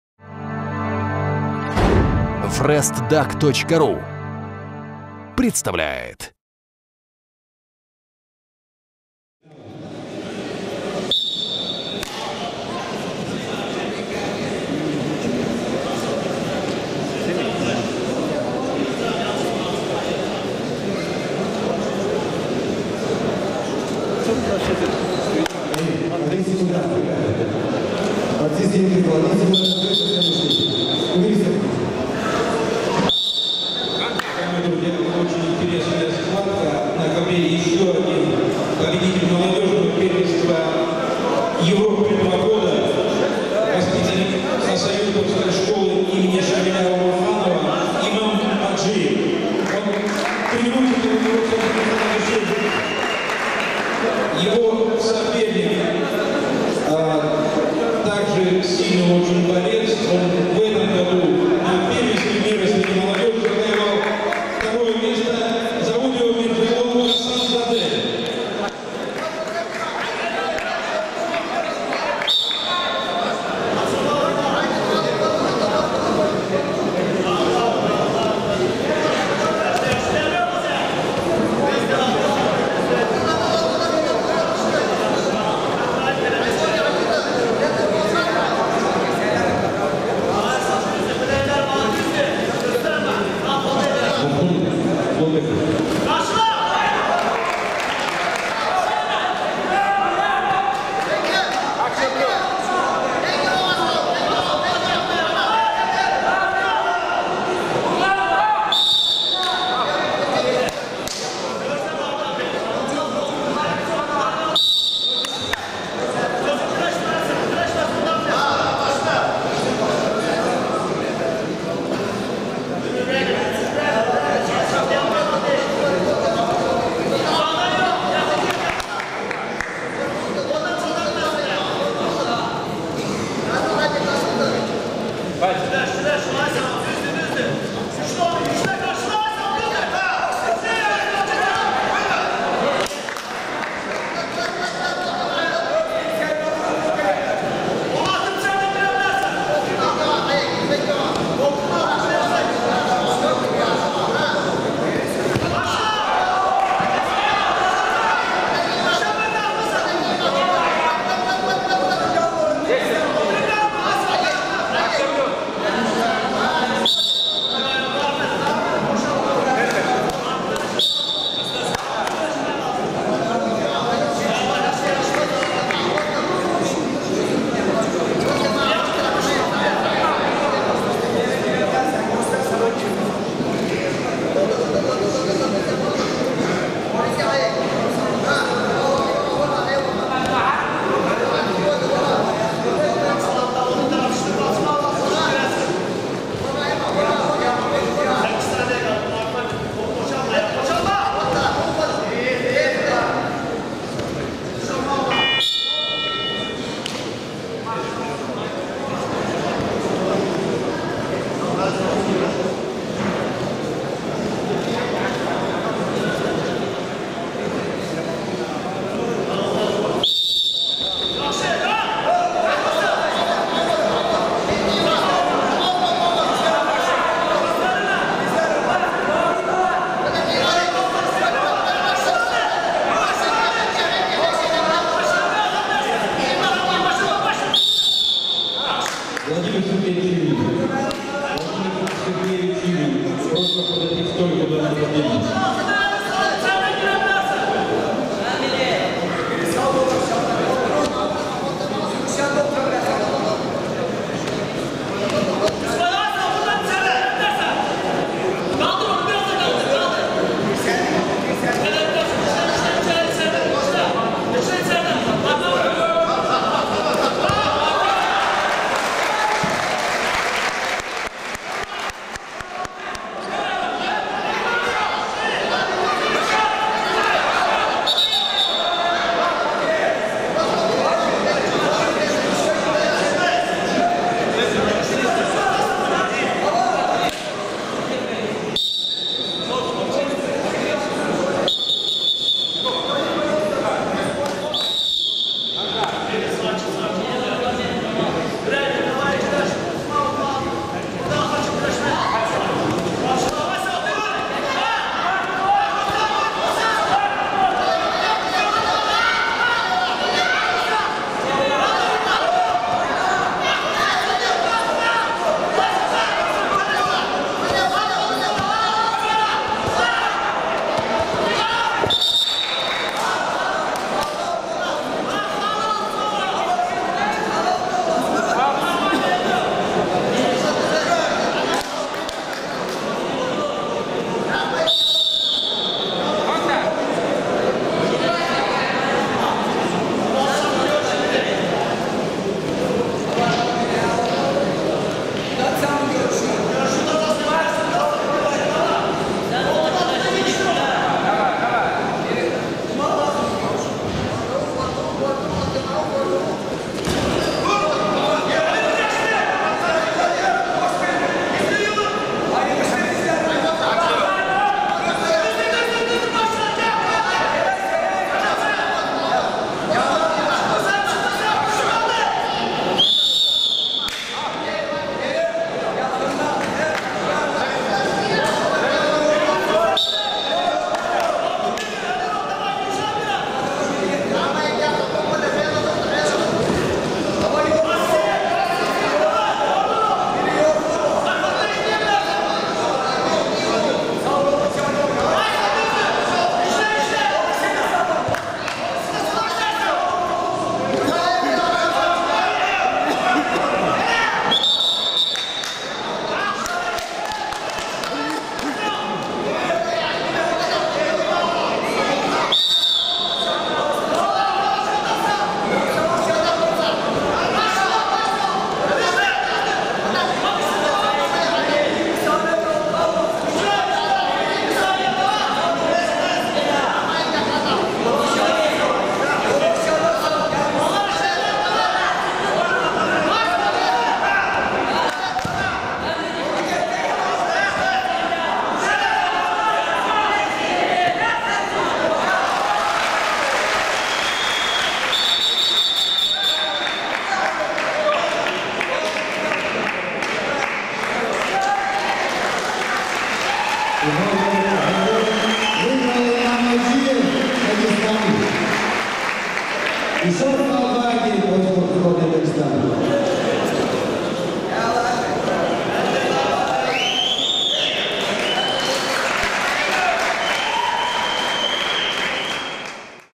Мемориал Омарова-2014_60 кг Гасанзаде-Аджиев
Турнир памяти Магомеда Омарова среди юниоров. Каспийск, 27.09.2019.